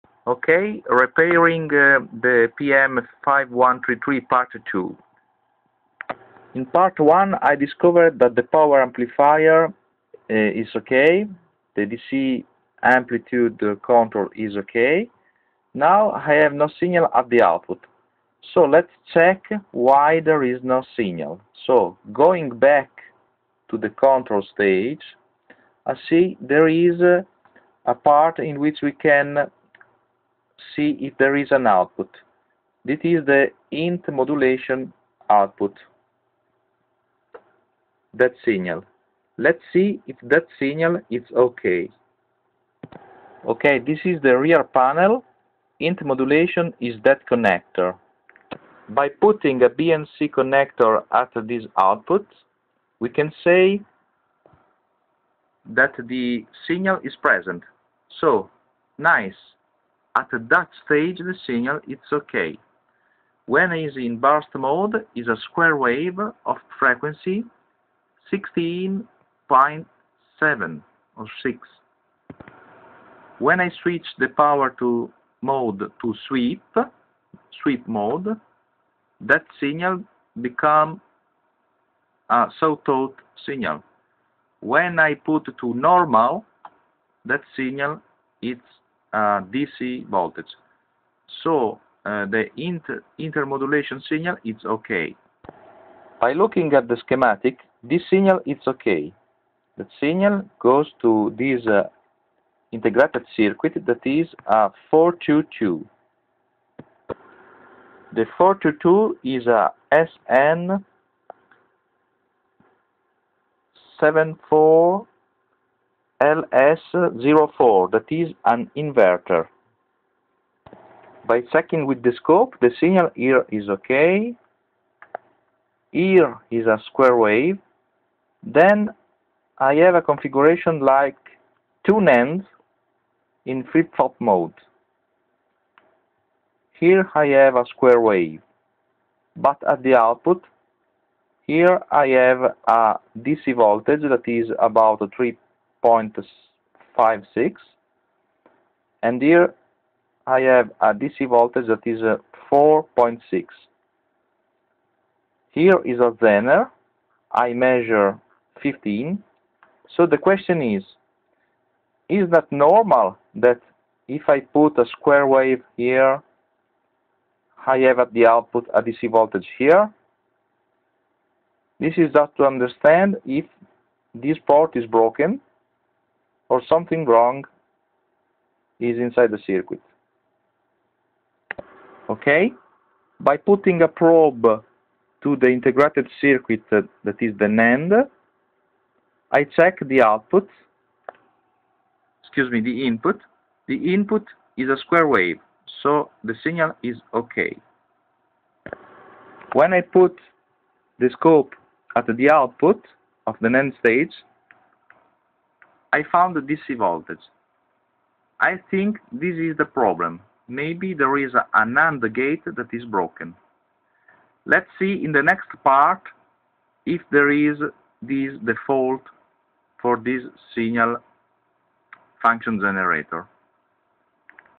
0102: PHILIPS PM5133S Function Generator - Teardown and repair (Part 2)
By going back from the output up to the INT modulation signal, maybe I found a fault. I'm suspecting that a NAND gate is not working properly. If the output of a NAND gate is connected to the anode of a zener diode, it should be propagate the square wave input. Let's simulate it and check if it's broken...